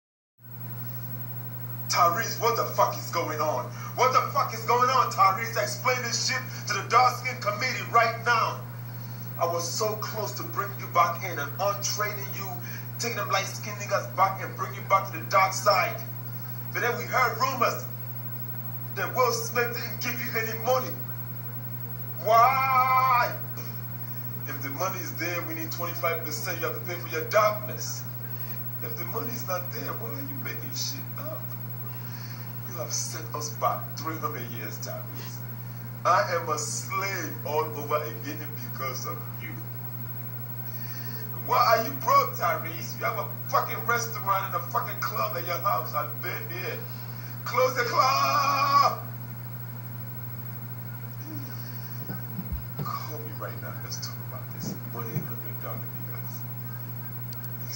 Michael Blackson Has Some More Words for Tyrese "Why Are You Broke, You have A Restaurant & Club"
Our SocialMedia:

Google+ @Imperialgiftpromotions